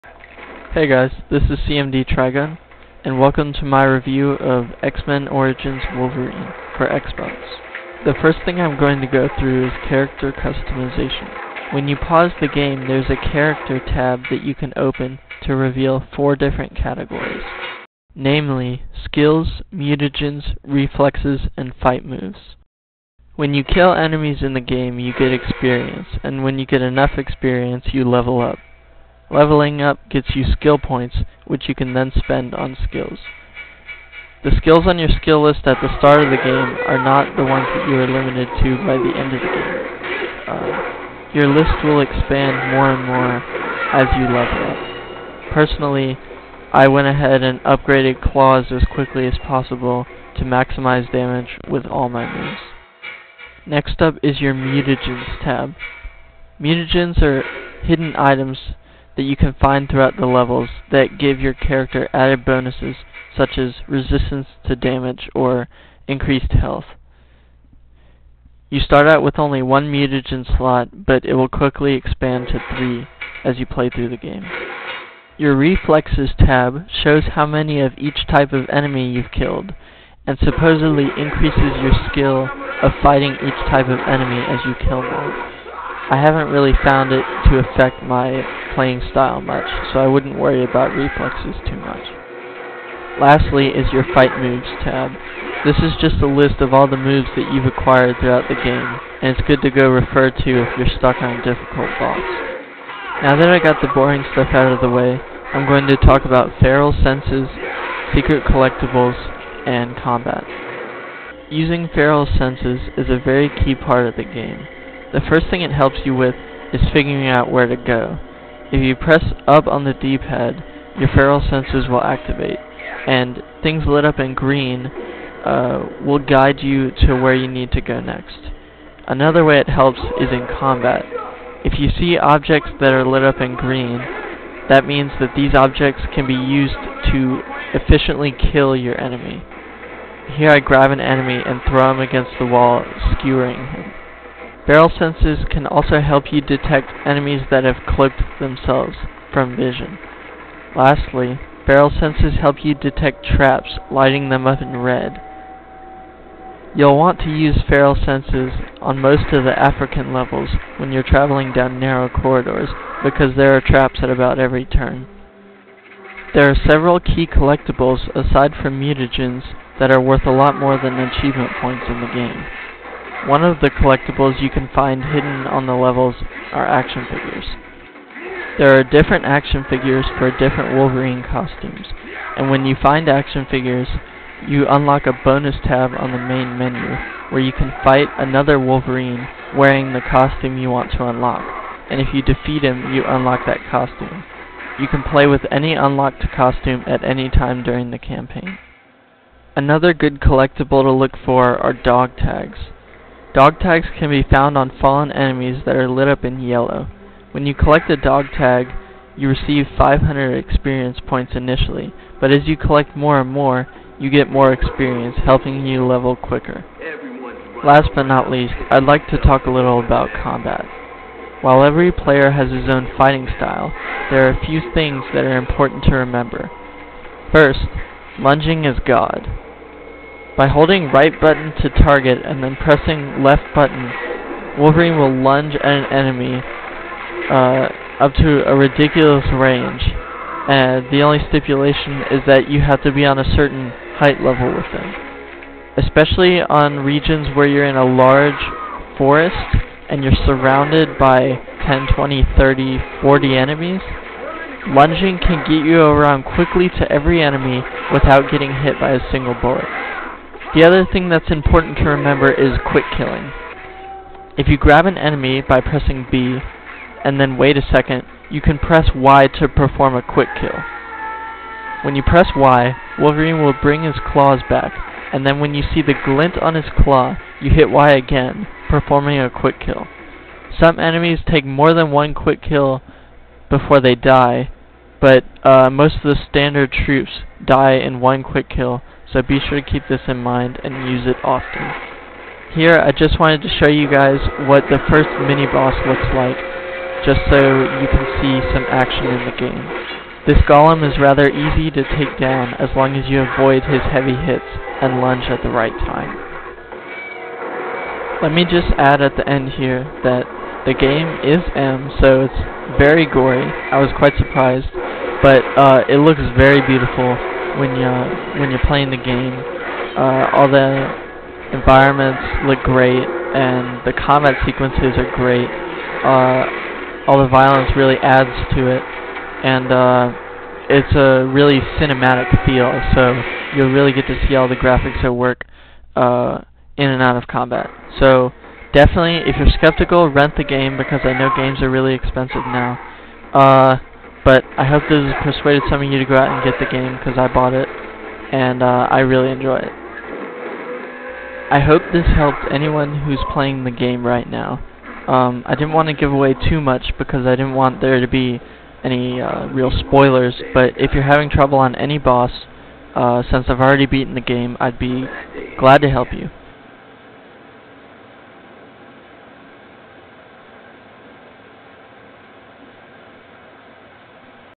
X Men Origins Wolverine Game Review
I sat down and plowed through this game in a few days so I could do a decent review for anyone playing the game! I can probably help you with any tricky puzzles or tough boss fights, especially if you are playing the Xbox 360 version like I did. I apologize for the less than great video quality, I still don't have a good capture card yet. Regardless, enjoy the review, I did put a lot of effort into it :)